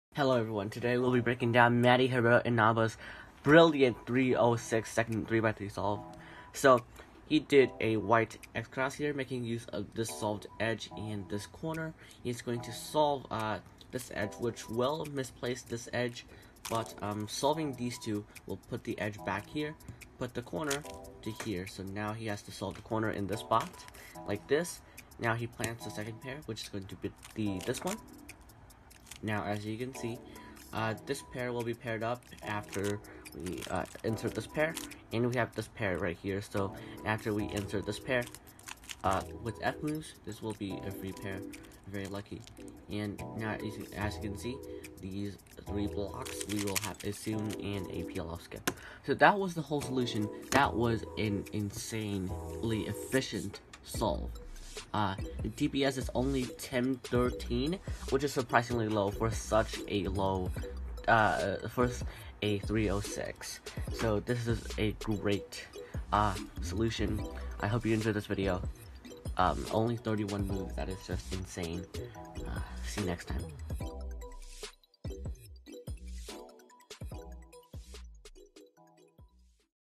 Matty's 31-move brilliance (although it only lasted 3.06 seconds)
What I meant was that it's better for his brilliance to last for a whole Ao5
Scramble: R' B2 R D2 L2 F R2 B' L F2 D F2 B2 D R2 U' F2 R2 B2 D2 R2